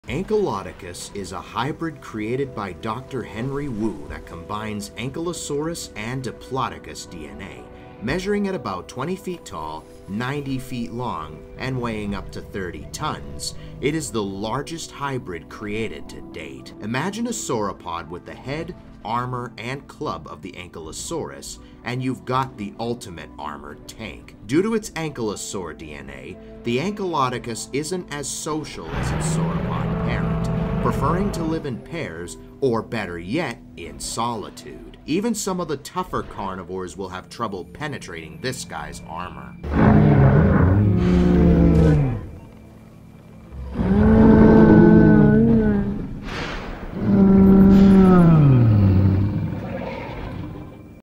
Random Dinosaur Facts in Under a Minute Ep 131: Ankylodocus
On today's episode of Random Dinosaur Facts, we will be talking about the hybrid known as Ankylodocus! A hybrid that combines DNA from Ankylosaurus & Diplodocus! Enjoy!